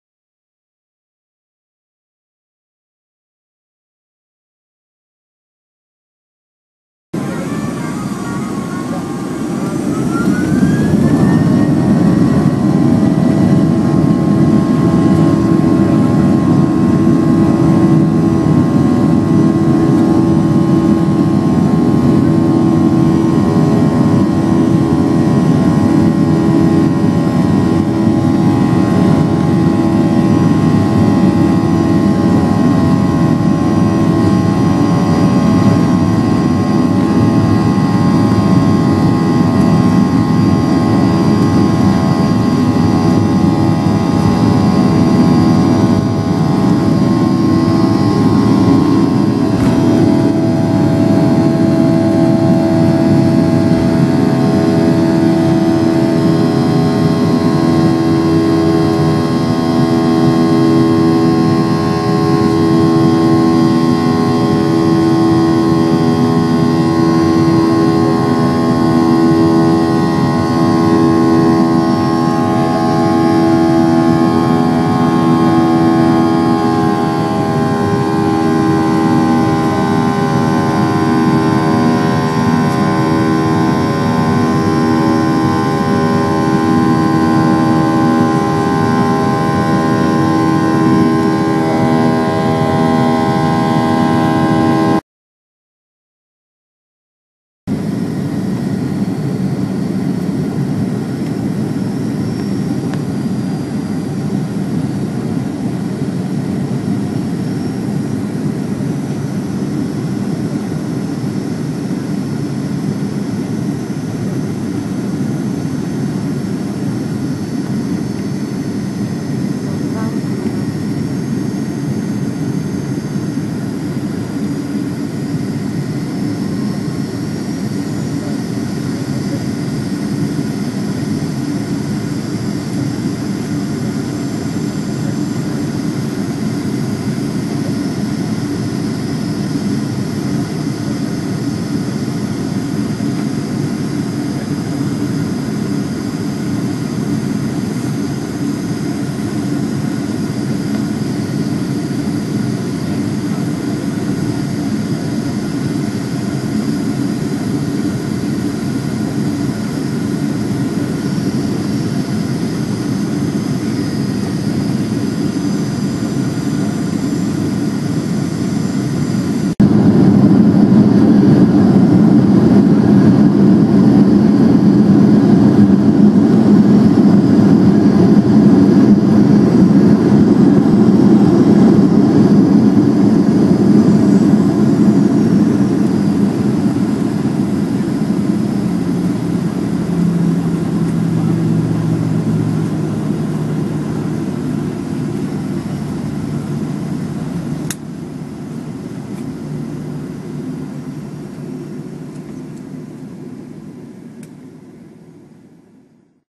Peter Rosenfeld takes off from Mumbai and lands at Kochi airport, India
On 9th November 2011 Peter Rosenfeld set off on a flight from Mumbai to Kochi in India. This video captures the two airports.